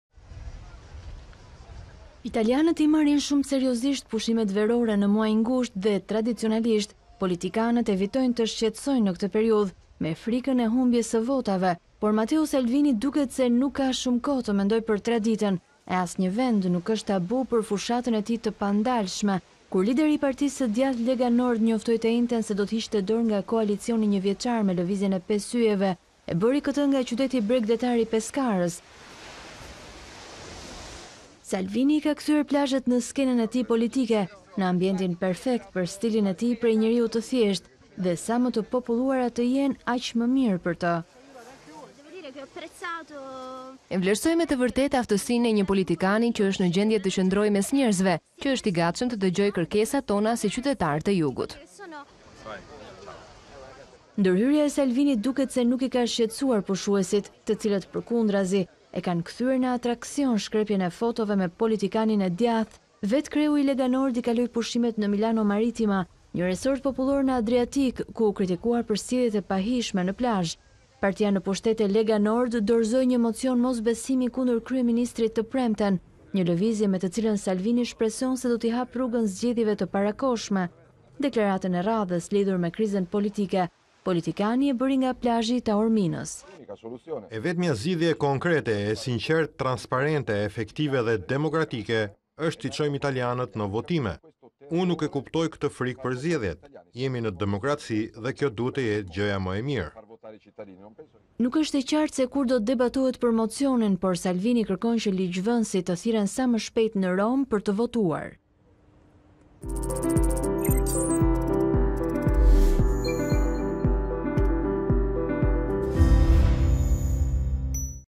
Salvini ‘‘pushton’’ plazhet, bën fushatë mes pushuesve në bregdet
Italianët i marrin shumë seriozisht pushimet verore në muajin gusht dhe tradicionalisht politikanët evitojnë të shqetësojnë në këtë periudhë, me frikën e humbjes së votave.
Por Matteo Salvini duket se nuk la shumë kohë të mendojë për traditën,  e asnjë vend nuk është tabu për fushatën e tij të pandalshme.
Kur lideri i partisë së djathtë “Lega Nord” (edhe pse fjalën Nord, që do të thotë Veri e kanë tashmë hequr zyrtarisht)  njoftoi të enjten se do të hiqte dorë nga koalicioni një vjeçar me lëvizjen e 5 yjeve, e bëri këtë nga qyteti bregdetar i Pescarës.
Salvini kishte përgatitur terrenin me një fjalim të zjarrtë një natë me herët në Sabaudia, një resort bregdetar në jug të Romës.
Salvini ka kthyer plazhet në skenën e tij politike, në ambientin perfekt për stilin e tij prej njeriut të thjeshtë, dhe sa më të populluara të jenë aq më mirë për të. Ndërhyrja e Salvinit duket se nuk i ka shqetësuar pushuesit, të cilët përkundrazi e kanë kthyer në atraksion shkrepjen e fotove me politikanin e djathtë që nuk thotë kurrë jo. Vetë kreu i Lega Nord i kaloi pushimet në Milano Marittima, një resort popullor në Adriatik, ku u kritika për sjelljet e pahijshme në plazh.
Partia në pushtet e Lega Nord dorëzoi një mocion mosbesimi kundër kryeministrit të premten, një lëvizje me të cilën Salvini shpreson se do t’i hapë rrugën zgjedhjeve të parakohshme. Deklaratën e radhës lidhur më krizën politike, politikani e bëri nga plazhi i Taorminës.
“E vetmja zgjidhje konkrete, e sinqertë, transparente, efektive dhe demokratike është t’i çojmë italianët në votime. Unë nuk e kuptoj këtë frikë për zgjedhjet. Jemi në demokraci dhe kjo duhet të jetë gjëja më e mirë”, deklaroi Salvini.
Nuk është e qartë se kur do të debatohet për mocionin, por Salvini kërkon që ligjvënësit të thirren sa më shpejt në Romë për të votuar.